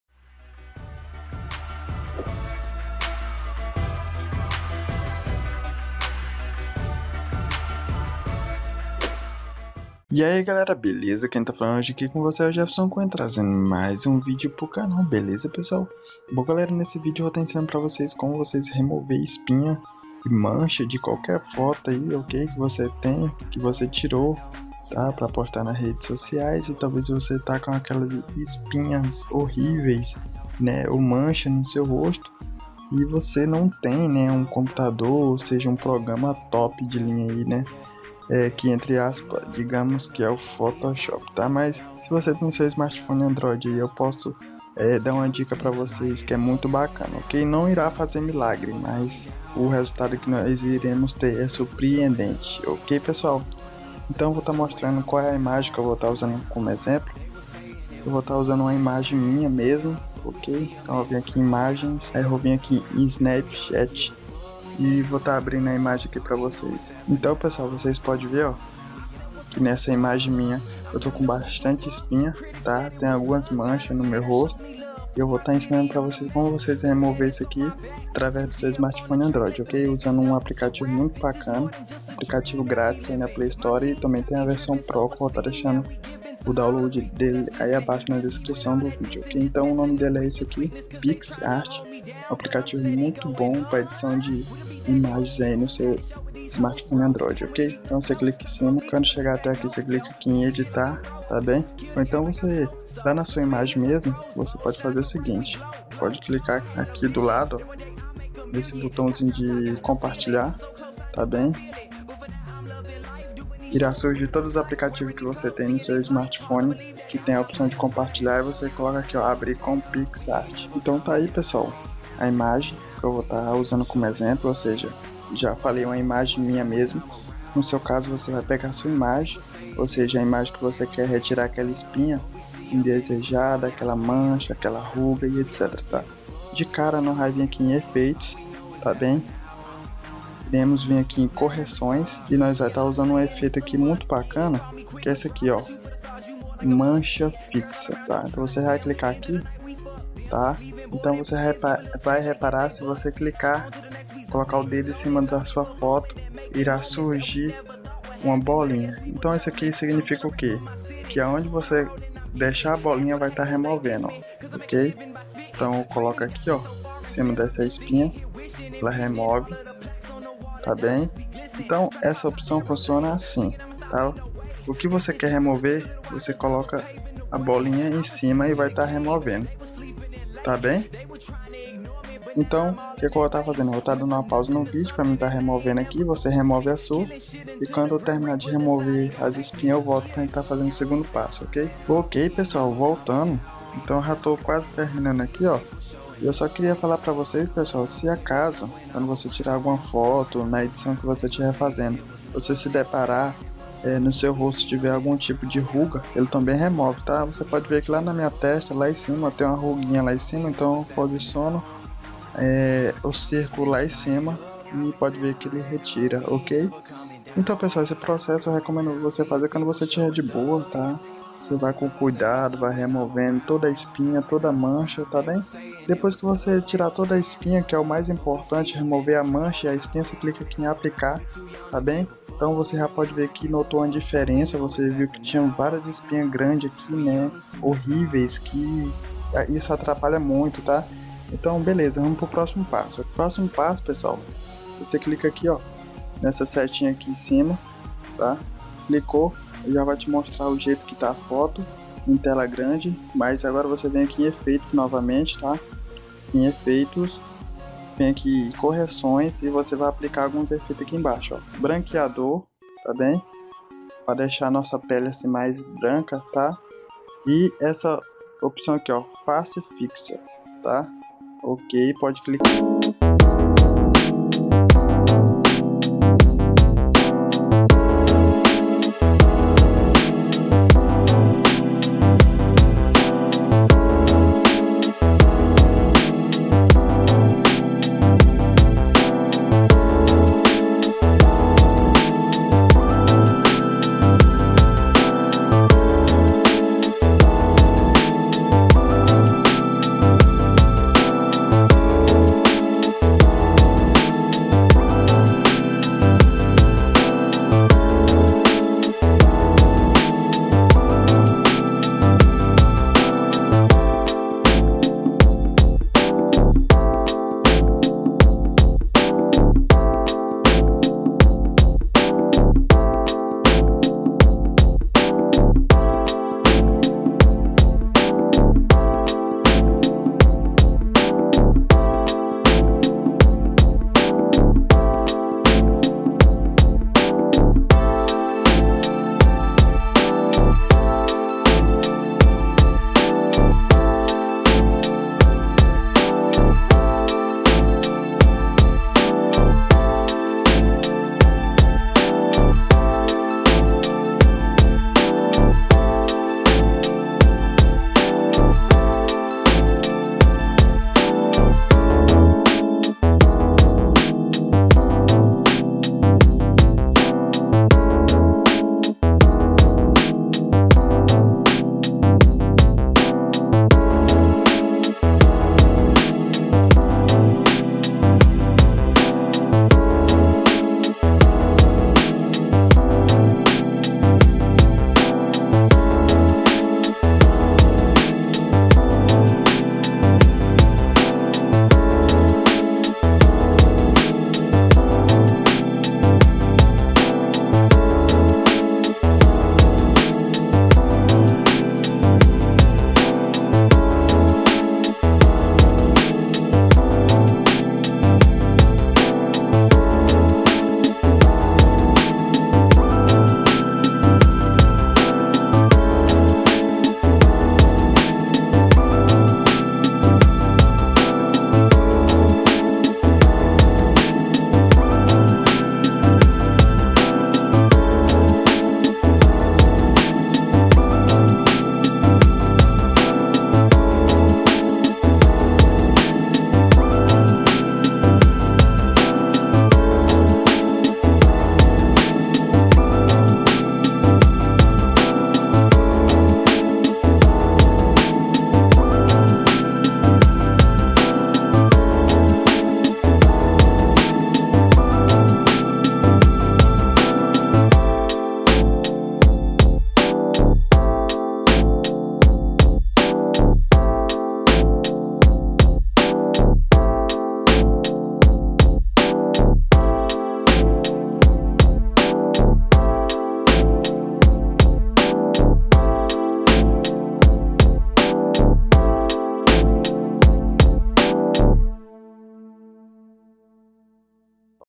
Tutorial - Como Remover Espinhas e Manchas Da FOTO Usando seu Celular ANDROID - Atualizado!
Link Para Baixar PicsArt – Editor de imagens (Google Play)

▼ Me add ai no instagram ▼